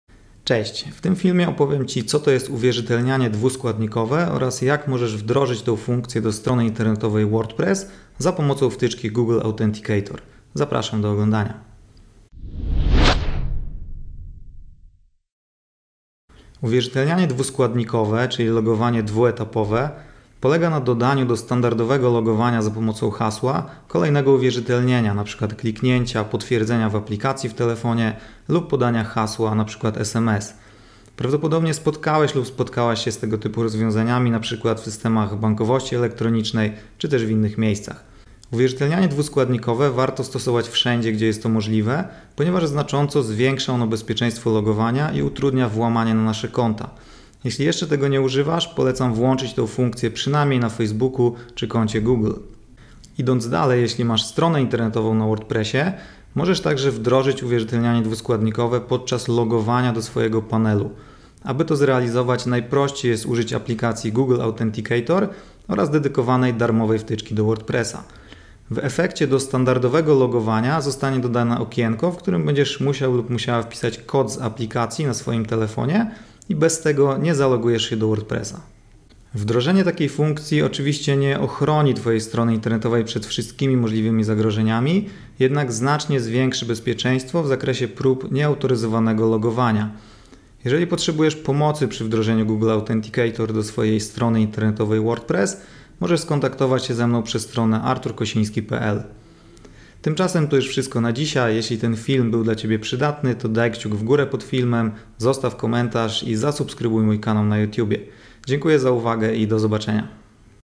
📲 WordPress + Google Authenticator (uwierzytelnianie dwuskładnikowe). Jak to działa?
Bezpieczeństwo w sieci jest (lub powinno być) priorytetem zarówno właścicieli stron, jak i ich użytkowników. Wobec coraz sprawniejszych metod cyberprzestępców, musimy mieć pewność, że podczas logowania i innych operacji nasze dane są bezpieczne. Jednym z narzędzi, które zwiększają nasze szanse w starciu z potencjalnym zagrożeniem jest uwierzytelnianie dwuskładnikowe, które na stronach internetowych może być realizowane przez wtyczkę Google Authenticator.

Co to jest uwierzytelnianie dwuskładnikowe? 🔒 Jak wdrożyć taką funkcję na stronie internetowej WordPress? Dowiedz się z mojego nowego filmu. 🍿

Potrzebujesz pomocy we wdrożeniu tego narzędzia? Skontaktuj się ze mną 📨